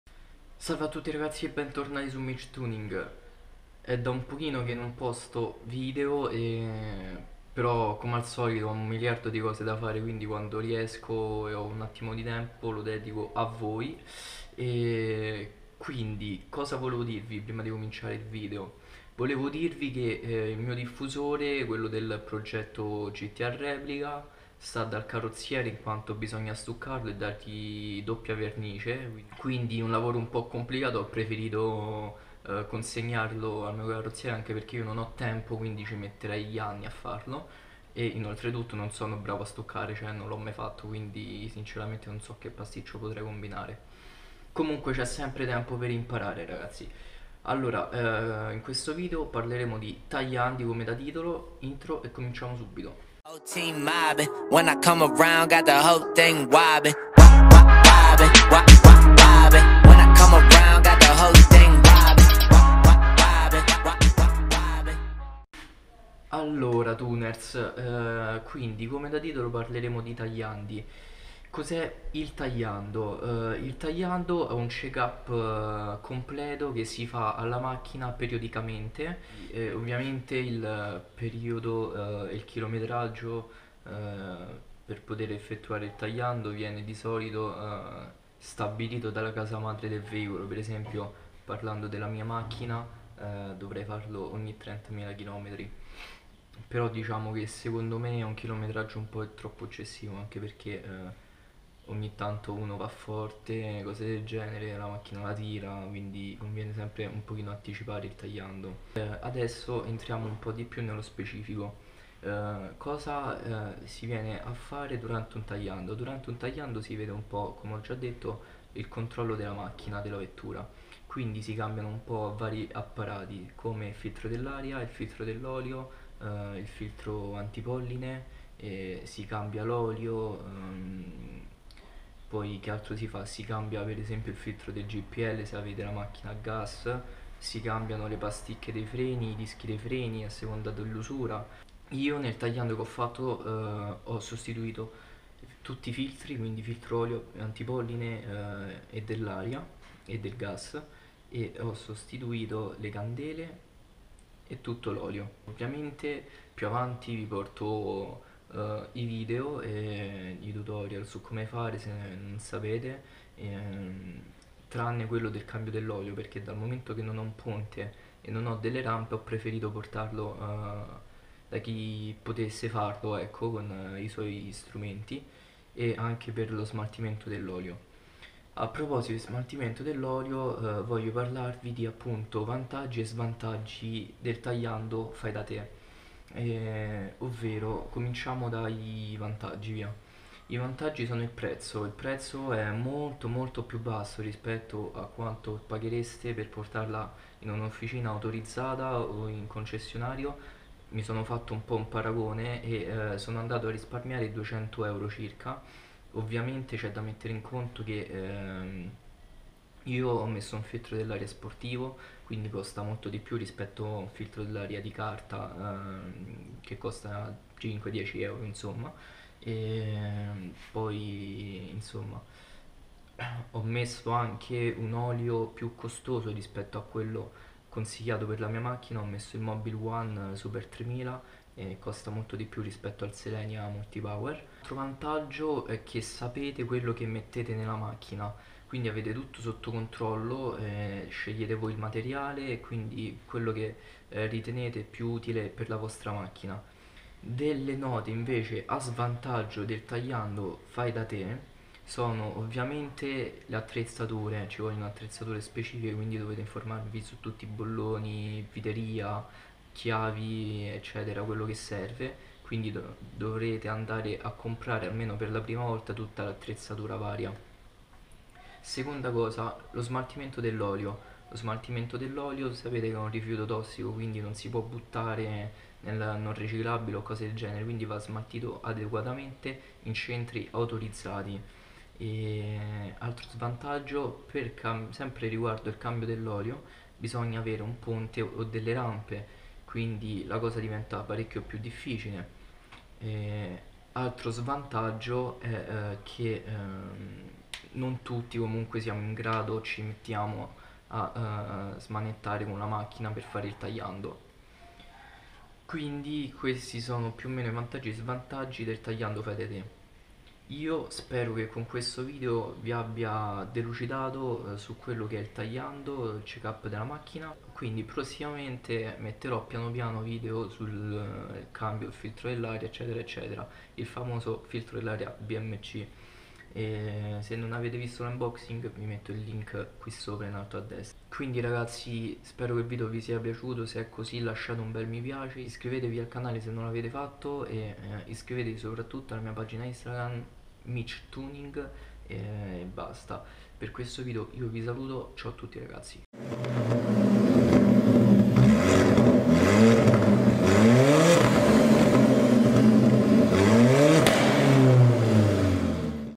Cos'è il tagliando? [Tagliando Alfa Romeo MiTo] #1
Salve a tutti ragazzi e bentornati sul canale, oggi inauguro con questa breve spiegazione i tutorial riguardo il tagliando.
Spero che l'iniziativa vi piaccia e che queste info vi siano utili.